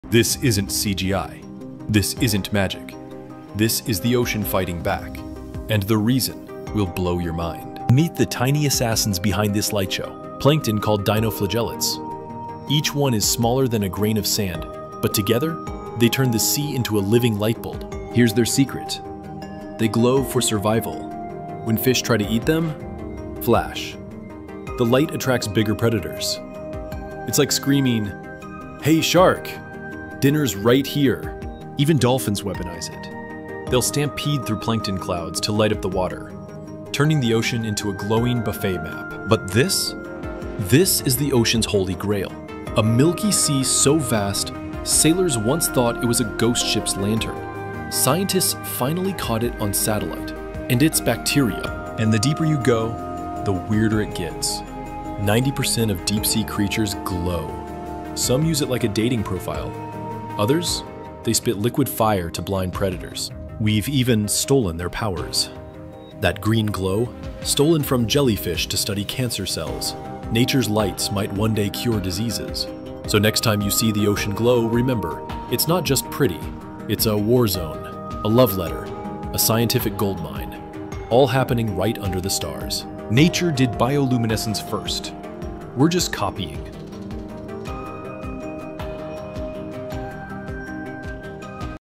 The Ocean Glows Like This… But Why?
Why Does the Ocean GLOW at Night?

Discover the fascinating science behind why the ocean glows at night in this captivating video. Learn about the natural phenomenon of bioluminescence, how marine organisms like plankton, jellyfish, and algae emit light, and where in the world you can witness this magical glow. From glowing waves to sparkling beaches, this video explores the causes, locations, and science of ocean bioluminescence. Perfect for science lovers, nature enthusiasts, and curious minds, this video answers all your questions about glowing seas. Watch now to uncover the mystery of why the ocean lights up after dark.

Let me know what you think about this in comments - I will try to reply and answer questions.